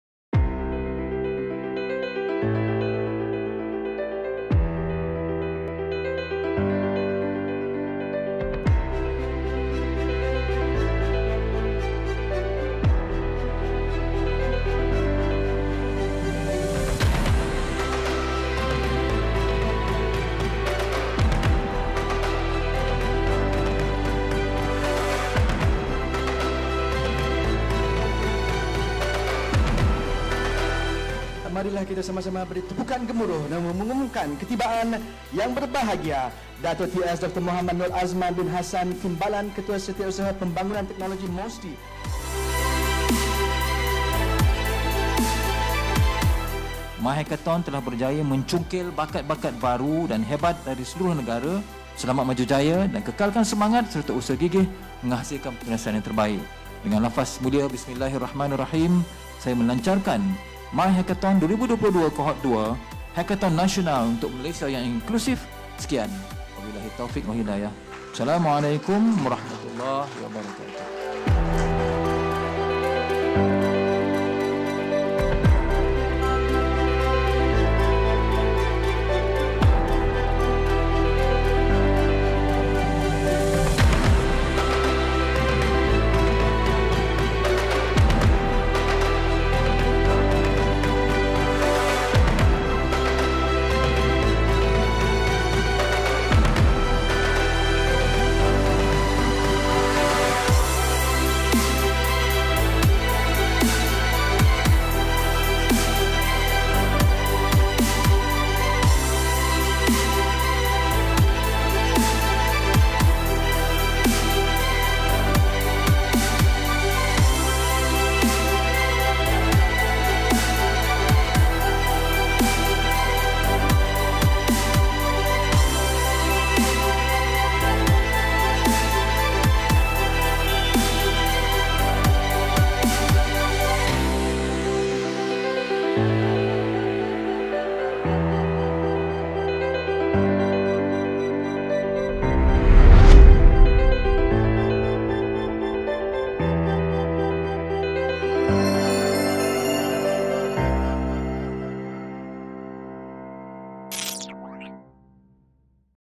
Majlis Pelancaran MYHackathon 2022 Cohort 2
MYHackathon 2022 Kohort Kedua kini telah dilancarkan secara rasmi!

Majlis pelancaran telah disempurnakan oleh YBhg Datuk Ts. Dr. Mohd Nor Azman Hassan, Timbalan Ketua Setiausaha (Pembangunan Teknologi) Kementerian Sains, Teknologi dan Inovasi (MOSTI). Majlis telah berlangsung dengan jayanya di Auditorium TEGAS Digital Village, Kuching, Sarawak.

Untuk Kohort Kedua ini, bakal peserta akan menyelesaikan 5 pernyataan masalah yang telah dikemukakan oleh kementerian dan agensi kerajaan, di mana pemenang akan dapat merealisasikan solusi pintar mereka sehingga ke peringkat projek rintis.

Kini bermulalah cabaran digitalisasi penyampaian perkhidmatan kerajaan.

Selamat maju jaya buat semua bakal peserta!